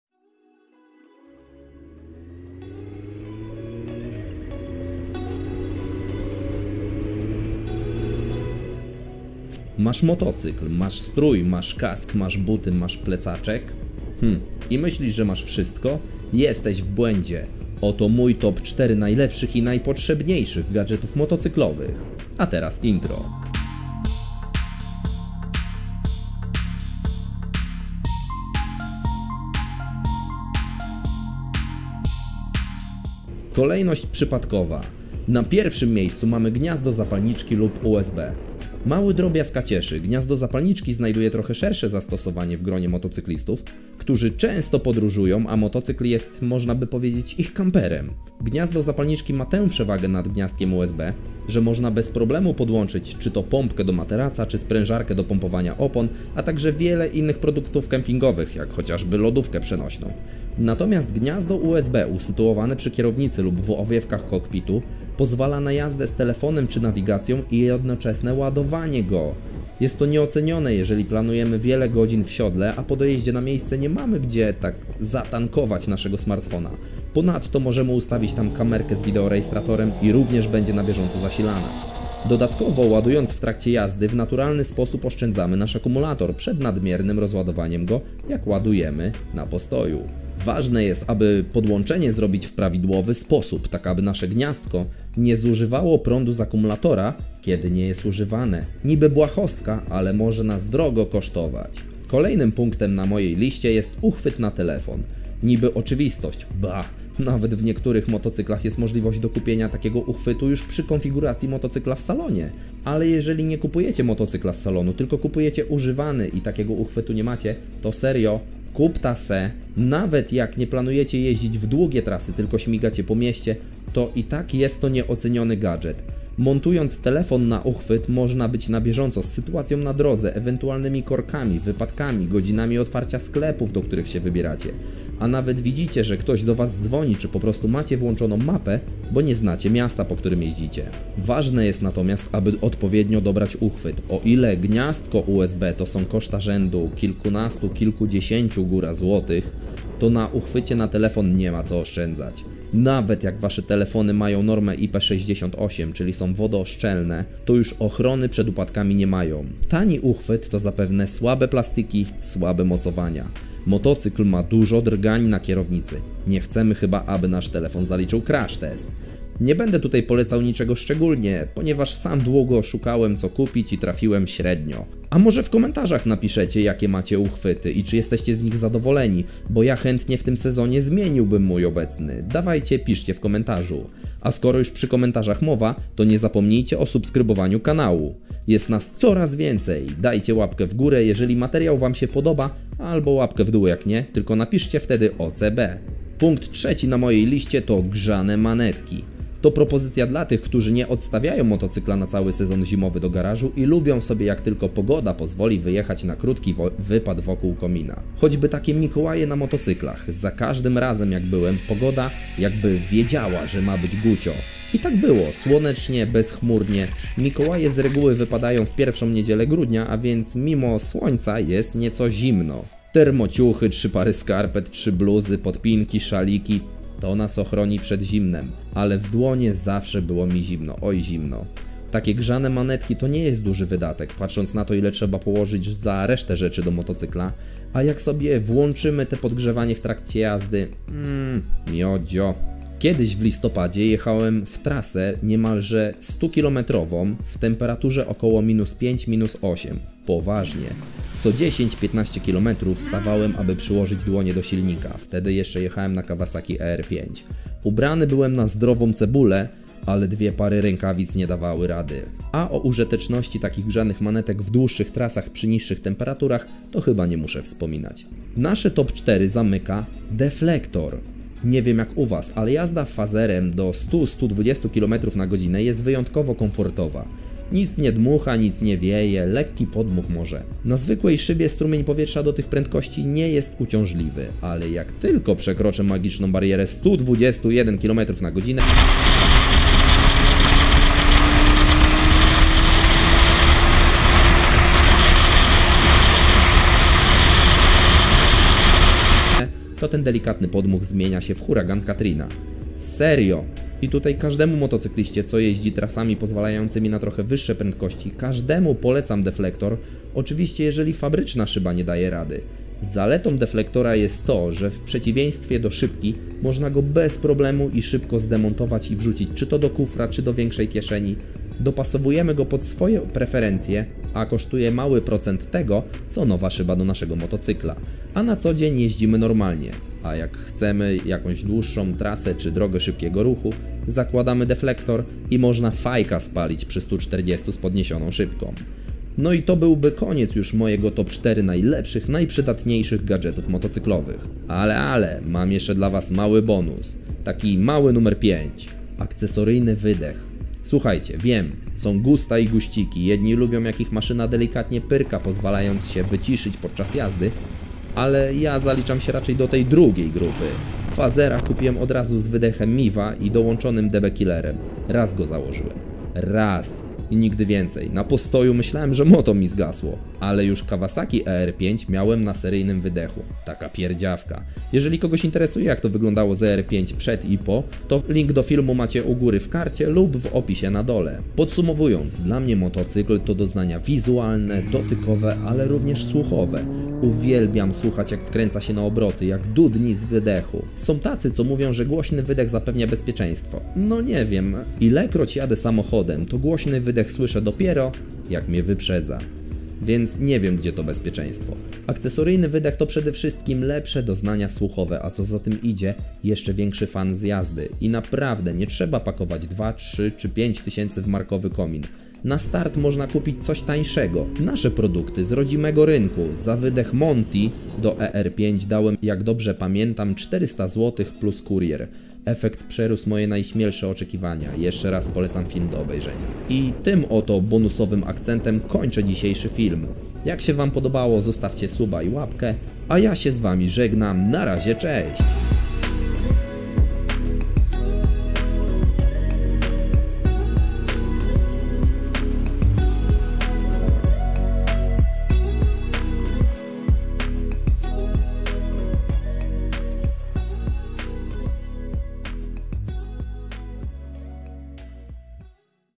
TOP 4 najlepszych, najprzydatniejszych gadżetów motocyklowych + BONUS
Cześć!

W tym filmie przedstawię Wam mój TOP 4 gadżetów motocyklowych.
Jest tego oczywiście dużo więcej, ale to temat na kolejny film ;P

Jeżeli podoba Ci się moja praca to możesz mnie wesprzeć na Patronite

A może coś więcej motocyklach? a proszę

A może chcesz sam coś nagrywać?